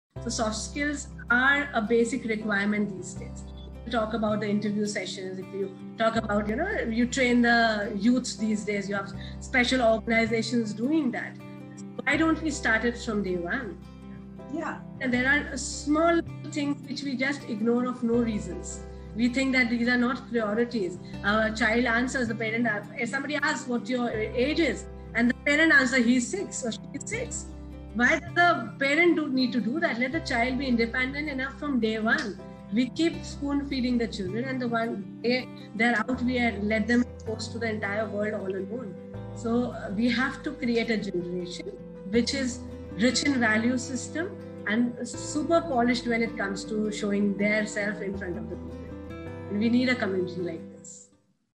Official Teaser-Ms.Monica Puri Principal – Seven Square Academy, Thane, Mumbai
We can't wait to share the nuggets of wisdom with all of you.. the exclusive interview with seemachhoriya featuring Ms.Monica Puri Principal – Seven Square Academy, Thane, Mumbai coming soon on youtube .